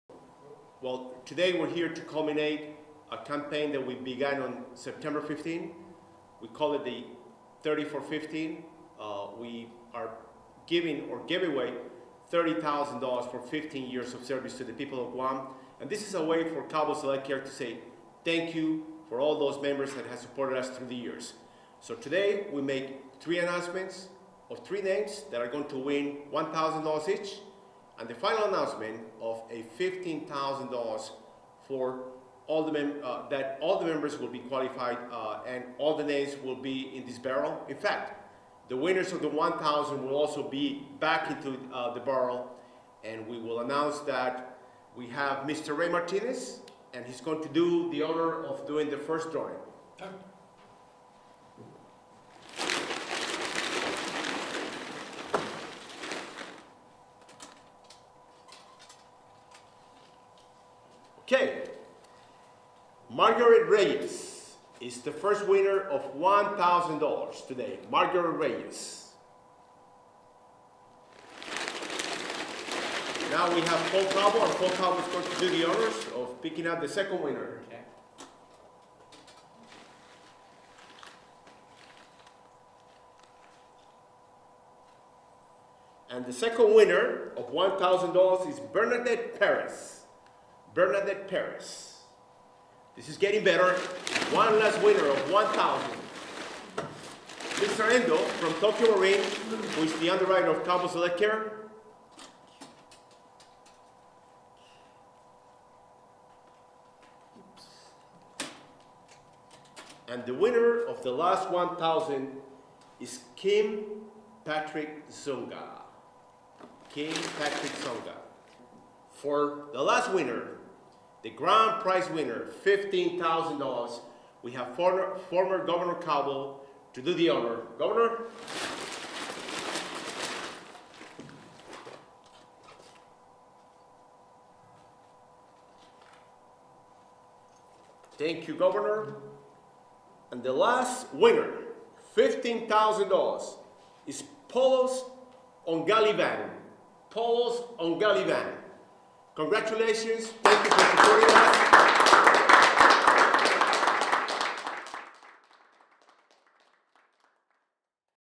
Calvo's announces $15,000 prize winner
Tuesday marked the culmination of the Calvo's SelectCare Anniversary Celebration, with the announcement of the lucky $15,000 grand prize winner. Executives from Calvo's SelectCare and underwriter Tokio Marine Insurance were on hand to pick the winners.